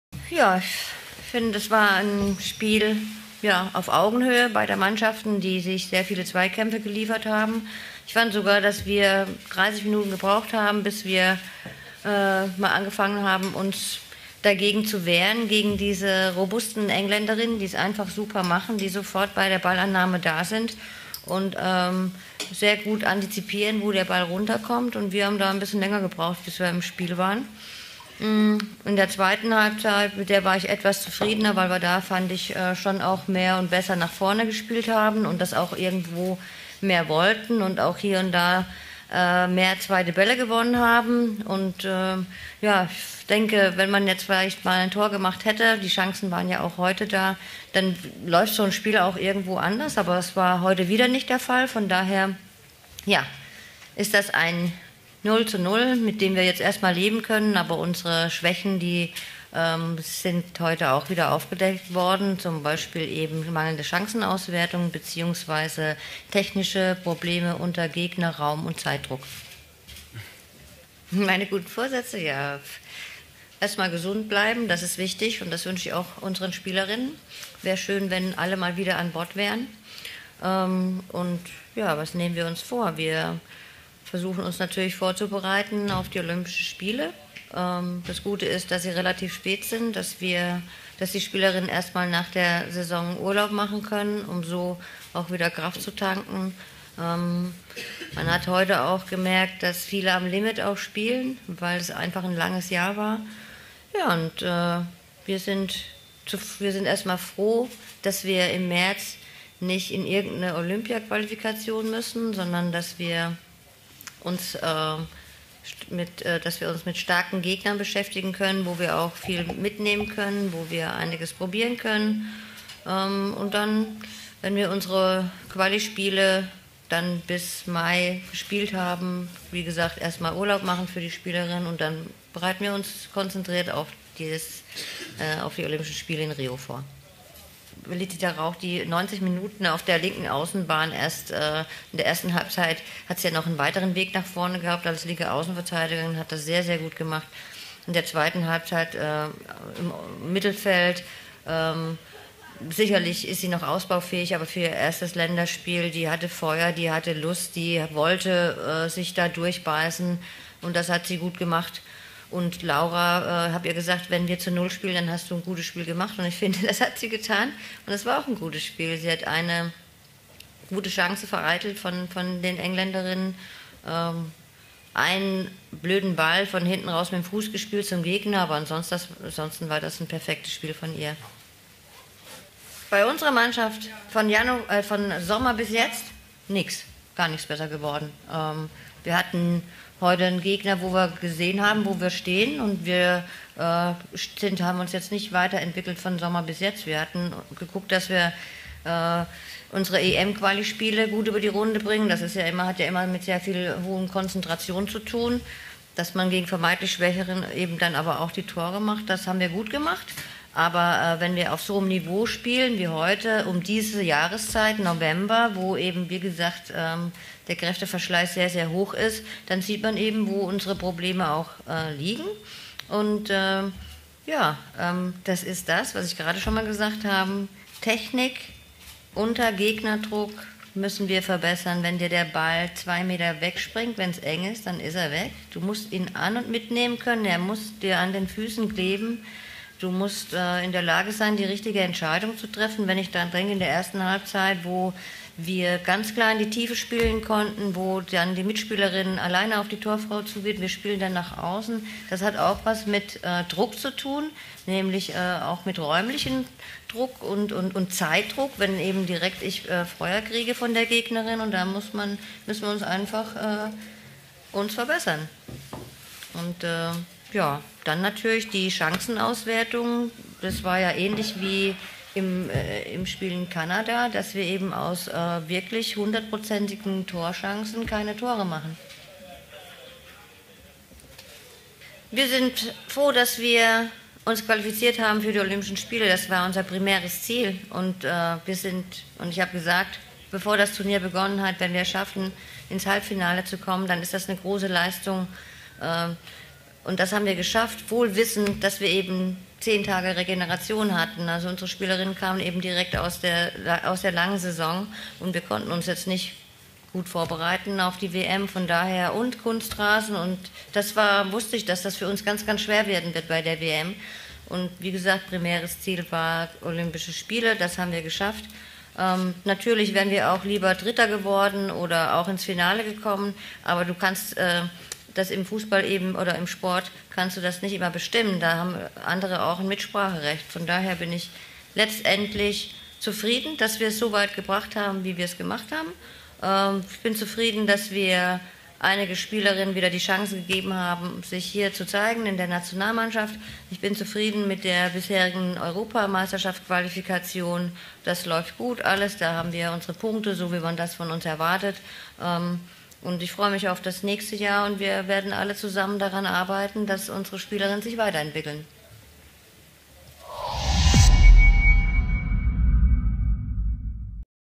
Silvia Neid: "Seit Sommer nicht weiter entwickelt"
Nach dem 0:0 im Testspiel gegen England zog Damen Bundestrainerin Silvia Neid auch eine kleine sportliche Bilanz für das Jahr 2015.